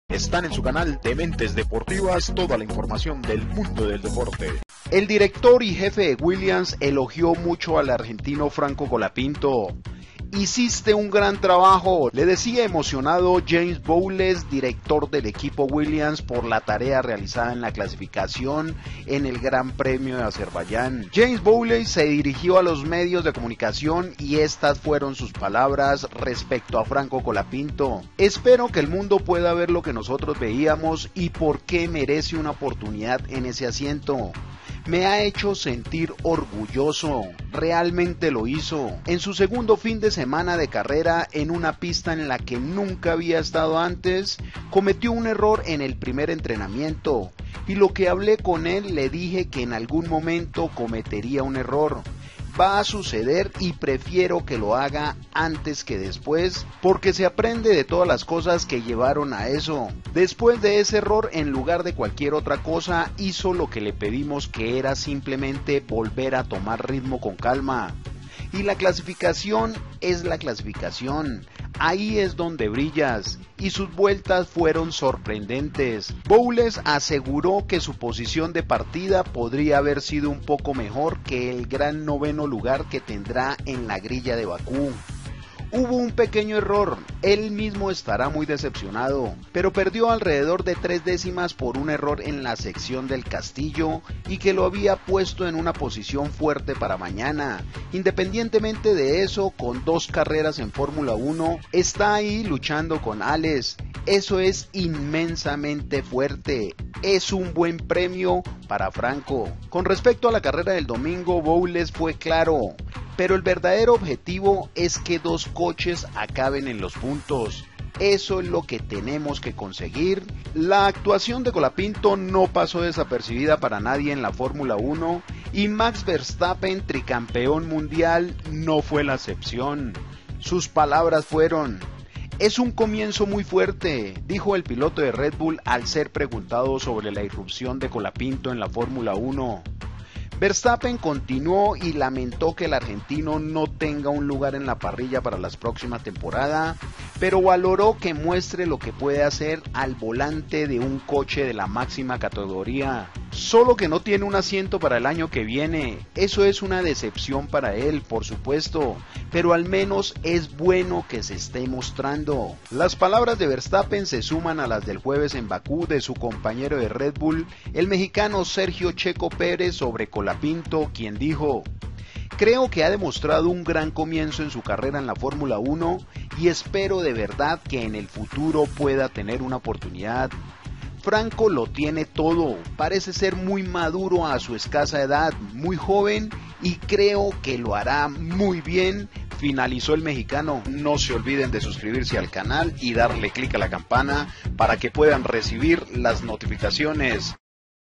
🔥 ASI REACCIONA JEFE de WILLIAMS a CLASIFICACION de FRANCO COLAPINTO GP AZERBAIYAN FORMULA 1
el director de Williams, elogió mucho al argentino. "¡HICISTE UN GRAN TRABAJO!". Le Decía emocionado James Vowles  director del equipo Williams por la tarea realizada en la clasificación a Franco colapinto ,